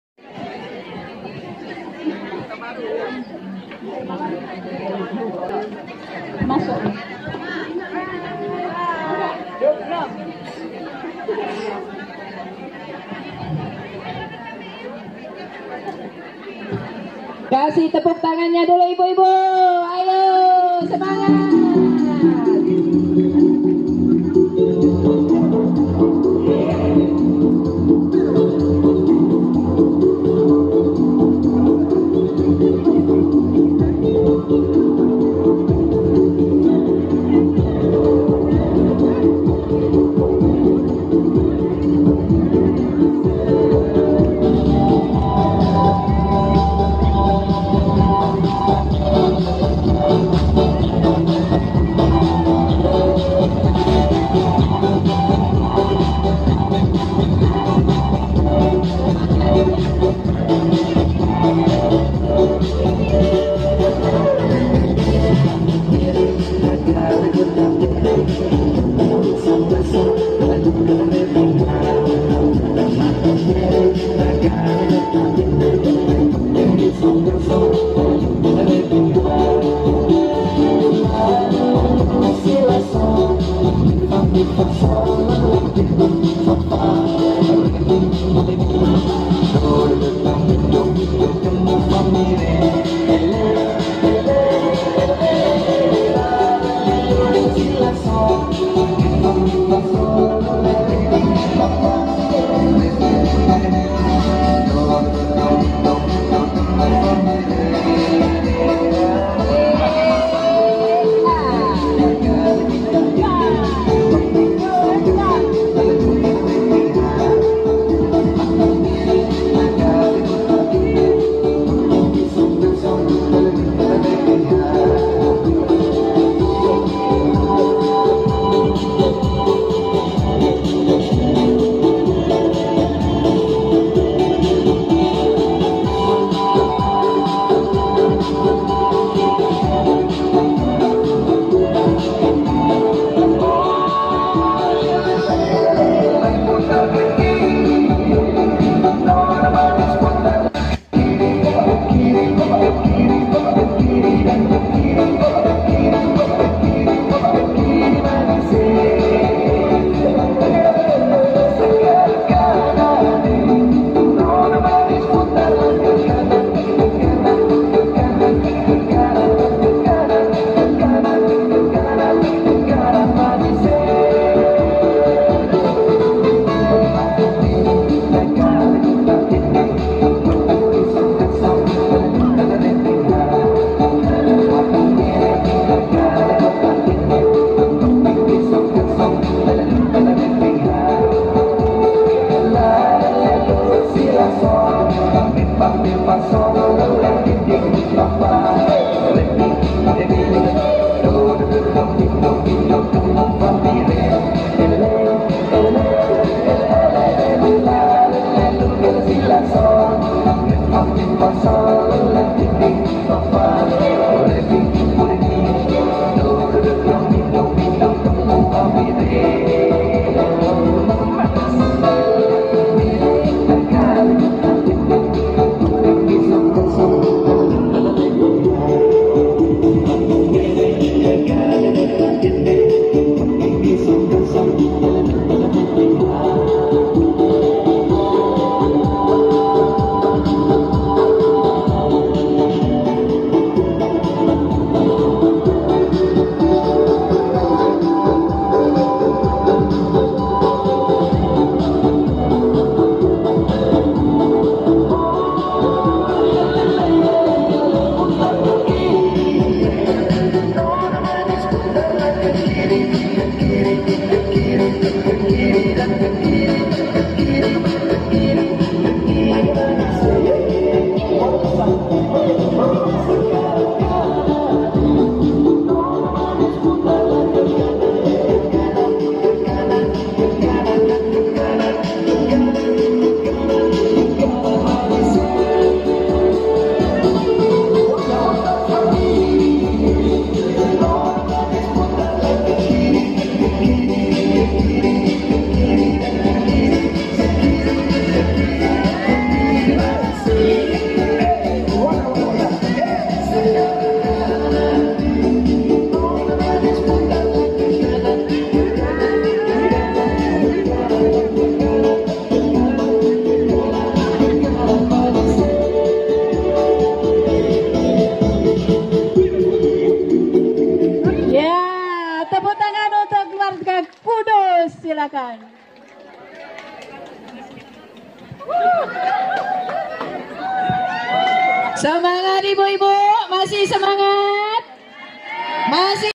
LOMBA SENAM MAUMERE - Peserta Dari Wanita Katolik RI DPC Paroki Keluarga Kudus Pontianak
Lomba Senam Maumere - STT Pontianak, 10 Juni 2018
Peserta Dari Wanita Katolik RI
DPC Paroki Keluarga Kudus Pontianak